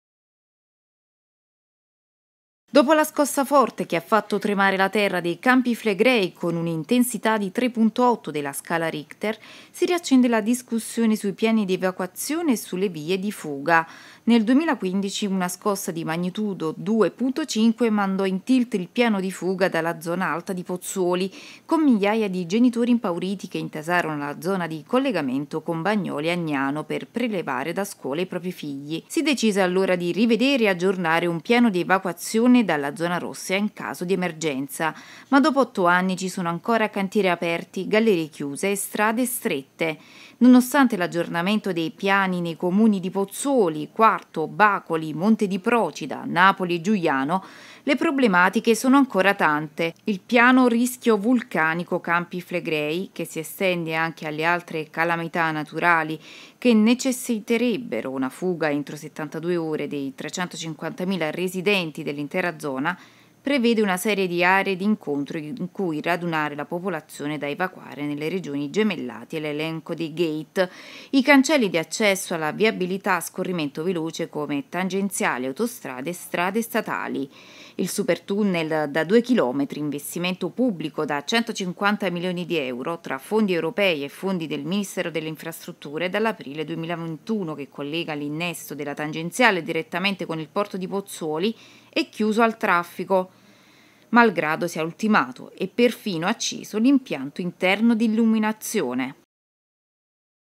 DOPO LA VIOLENTA SCOSSA DI TERREMOTO SI RIDISCUTE SUL PIANO DI EVACUAZIONE
Dopo la scossa forte che ha fatto tremare la terra dei Campi Flegrei con una intensità di 3.8 si riaccende la discussione sui piani di evacuazione e sulle vie di fuga. Nel 2015, una scossa di magnitudo 2.5 mandò in tilt il piano di fuga dalla zona alta di Pozzuoli, con migliaia di genitori impauriti che intasarono la zona di collegamento con Bagnoli e Agnano per prelevare da scuola i propri figli: si decise, allora, di rivedere e aggiornare un piano di evacuazione dalla zona rossa in caso di emergenza, ma dopo otto anni ci sono ancora cantieri aperti, gallerie chiuse e strade strette. Nonostante l’aggiornamento dei piani nei Comuni di Pozzuoli, Quarto, Bacoli, Monte di Procida, Napoli e Giugliano le problematiche sono ancora tante.